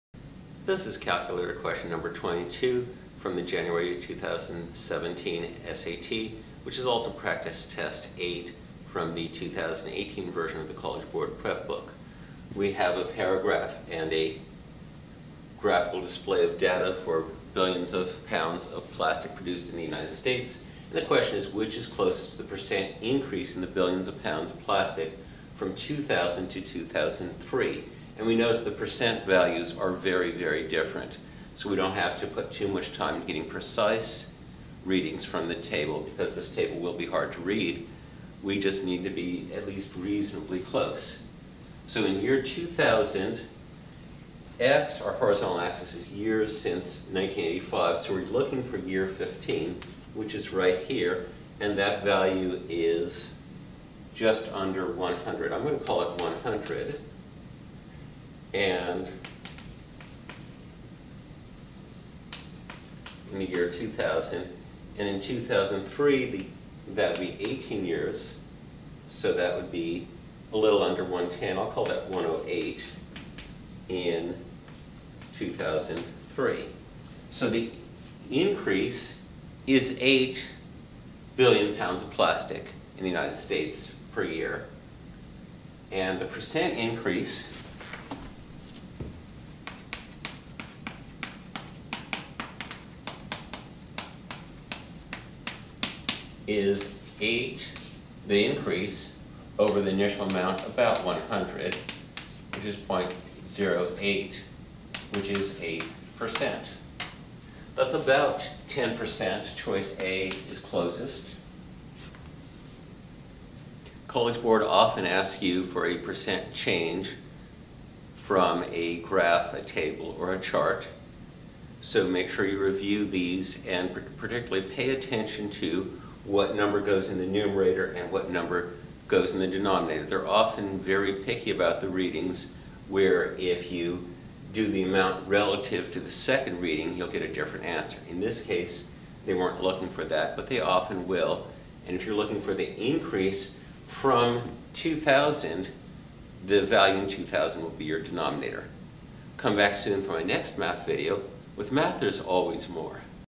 SAT Test 8: Interpreting graphs - question #4-22
Between 1985 and 2003, data were collected every three years on the amount of plastic produced annually in the United States, in billions of pounds. The graph below shows the data and a line of best fit. The equation of the line of best fit is y = 3.39x + 46.89, where x is the number of years since 1985 and y is the amount of plastic produced annually, in billions of pounds.
Which of the following is closest to the percent increase in the billions of pounds of plastic produced in the United States from 2000 to 2003?
A) 10%
B) 44%
C) 77%
D) 110%